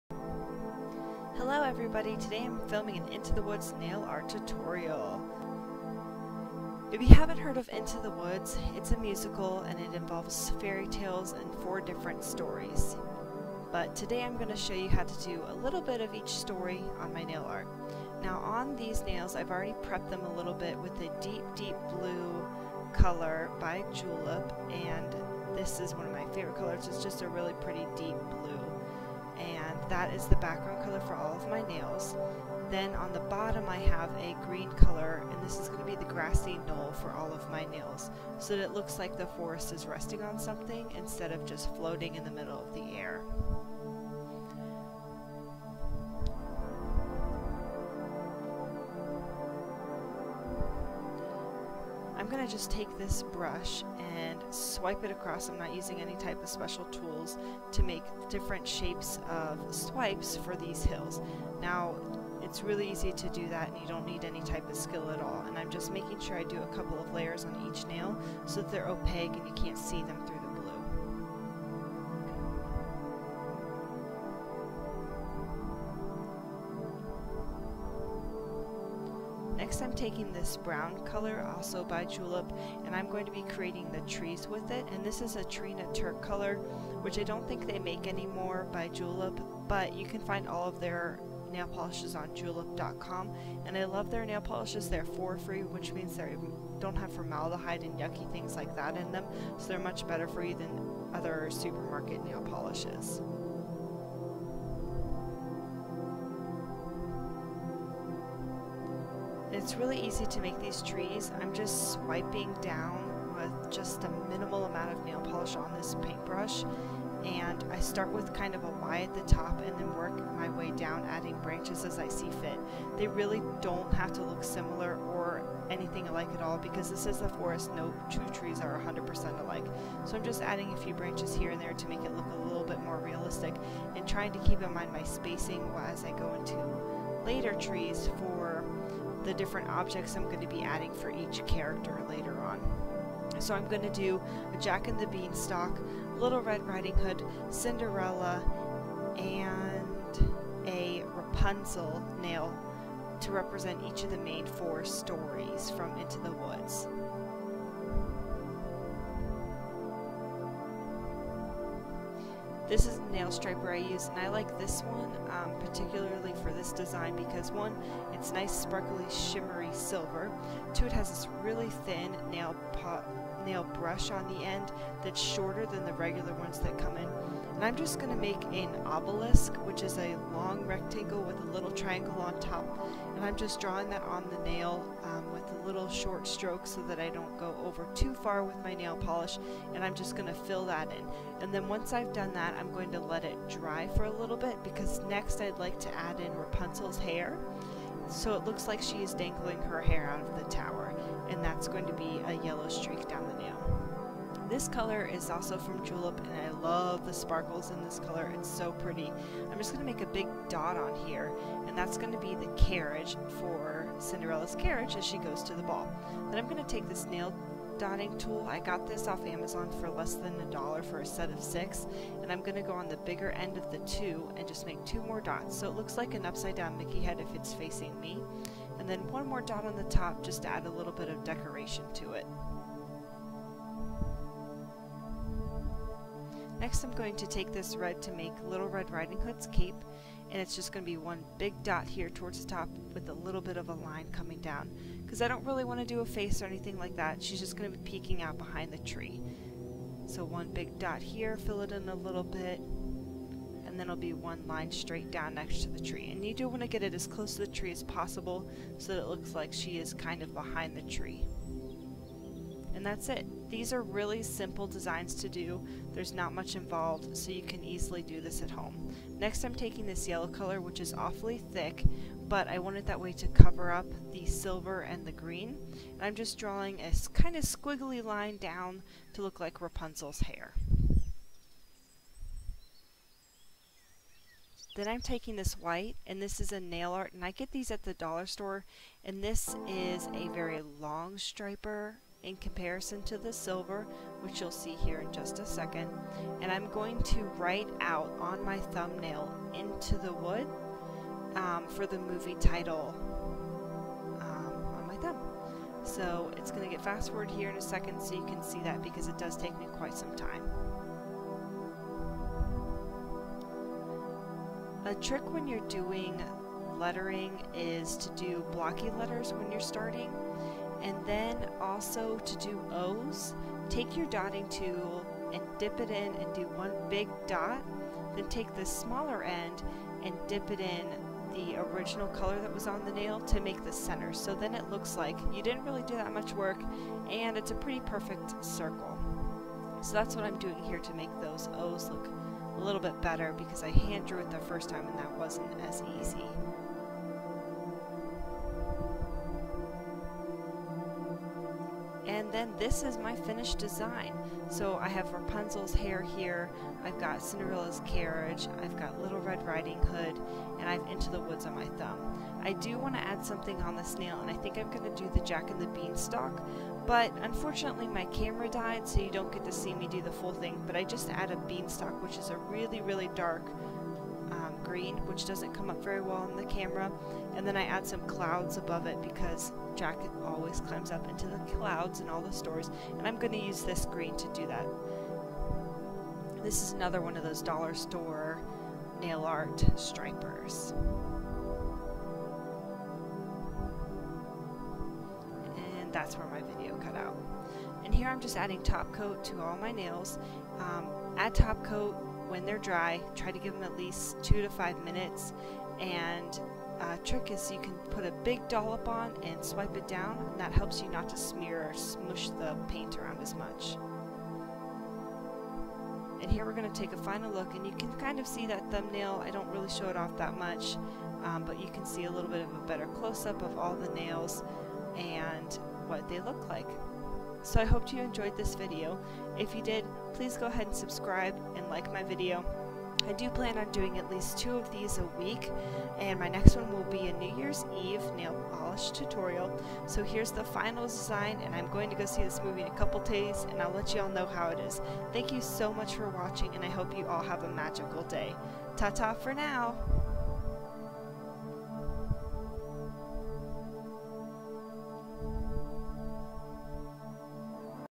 Into the Woods Nail Art Tutorial
Today I teach you how to do an Into the Woods themed nail! This nail art is so easy to do. I also do designs for Cinderella, Little Red Riding Hood, Jack and the Beanstalk, and Rapunzel.